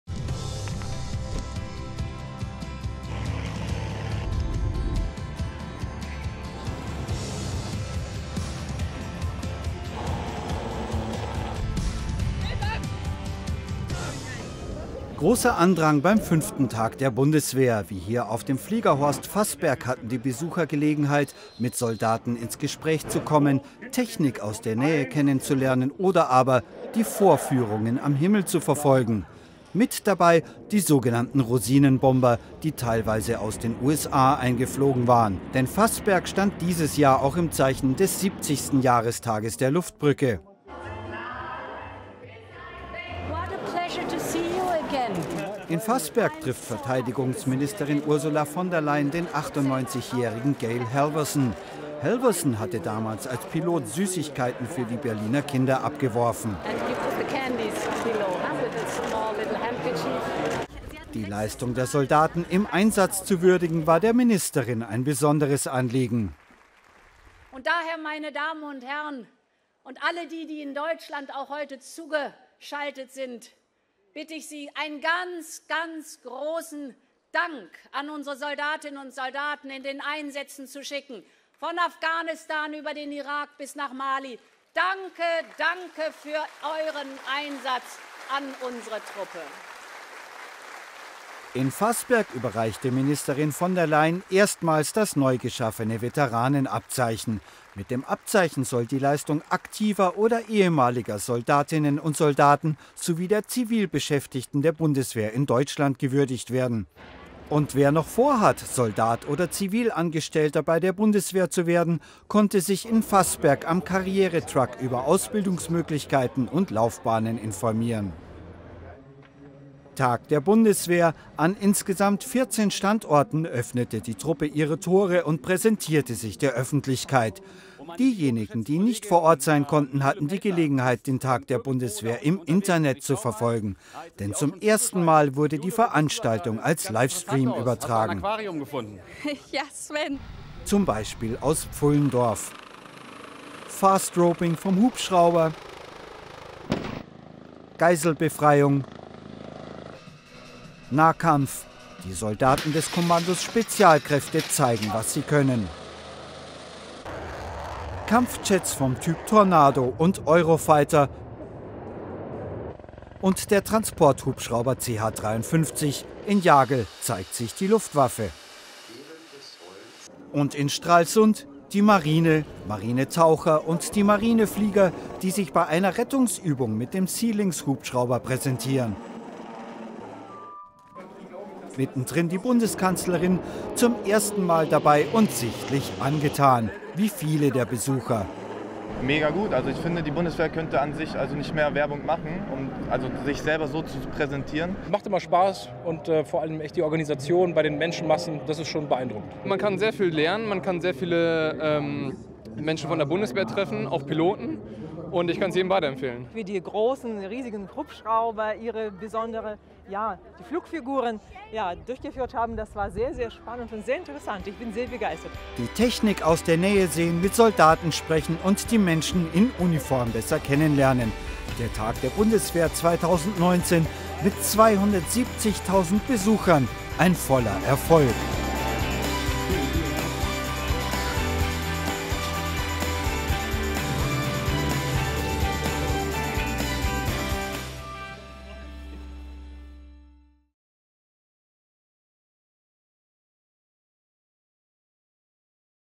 Das war der Tag der Bundeswehr 2019
270.000 Besucher kamen dieses Jahr zum fünften Tag der Bundeswehr. An 14 Standorten präsentierte sich die Bundeswehr in all ihren Facetten. Von Jagel bis Pfullendorf, von Münster bis Cham konnten die Besucher mit den Soldaten ins Gespräch kommen, Technik bestaunen und sich Vorführungen ansehen.
Ein besonderer Schwerpunkt war Faßberg mit einer Gedenkveranstaltung zum 70. Jahrestag der Luftbrücke. Faßberg war einer der Flugplätze, von denen damals im Minutentakt die „Rosinenbomber“ starteten, um Berlin aus der Luft zu versorgen

06/2019
19E20701